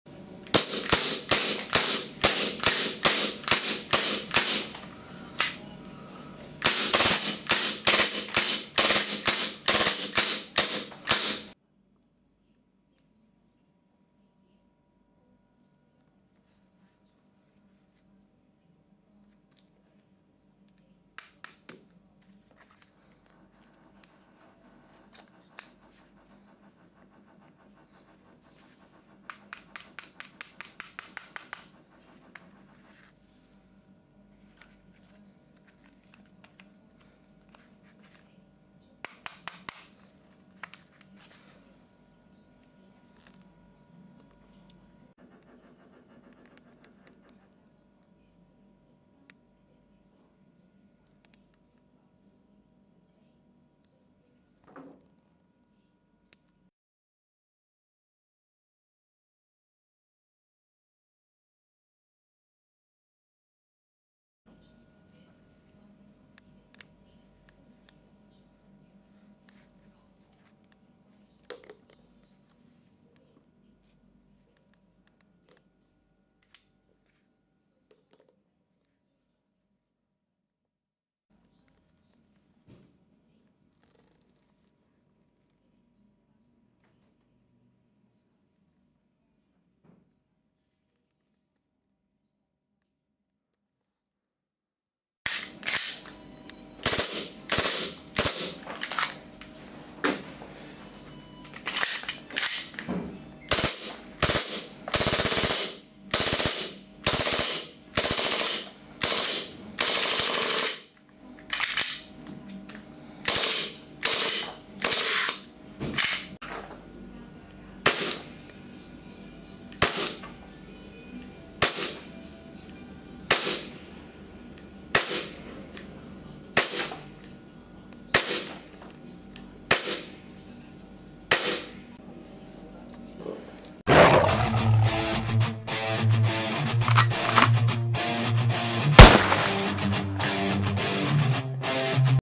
WE G34 / G35 Metal Slide GBB Pistol [Airsoft]
G34:
Is a competition version of G17 which fires the same round as G17
Metal Slide with "G34 Tactical" marking, discrete ejector and outer barrel.
Included 25 rounds G17 Magazine.

G35:
Is a competition version of G22.
Metal Slide with "G35 Tactical" marking, discrete ejector with outer barrel.
Compatible with WE 50rds Glock long mag and TM / Stark Arms G17/18 25 rds and 50rds long mag.